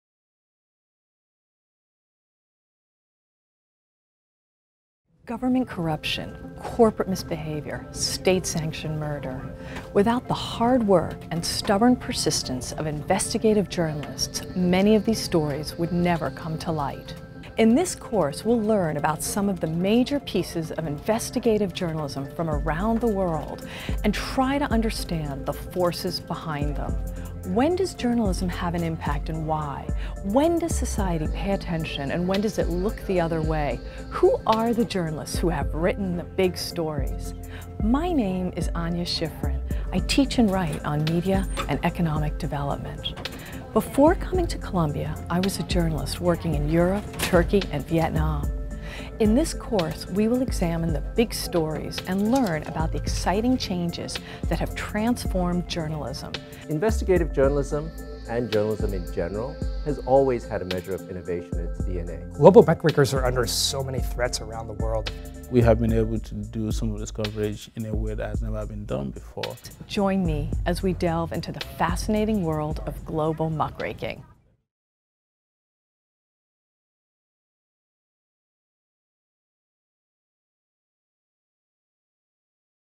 MOOC GM1x | Anya Schiffrin - Global Muckraking Investigative Journalism and Global Media | Trailer
Global Muckraking: Investigative Journalism and Global Media

Learn how brave journalists can rock the world by exposing corruption and human rights and labor abuses.

Using examples of investigative and crusading journalism from Asia, Africa, Latin America and Europe, this course will help you understand how raising public awareness can create political and social change.

This course is a fast-paced introduction to global muckraking, past and present, and includes penetrating interviews with historians and investigative journalists.

Join us to discover the vital role that journalism has played in fighting injustice and wrongdoing over the last 100 years and delve into the current trends reshaping investigative reporting in the digital age.

What you’ll learn:
How journalists can act as government and corporate watchdogs
The hard and soft pressures on investigative journalism
Stories of prominent reporters uncovering injustice from the late 19th century to today
Trends in media innovation

Start Date: January 18, 2017
End Date:  March 8, 2017
Certificate Date: March 10, 2017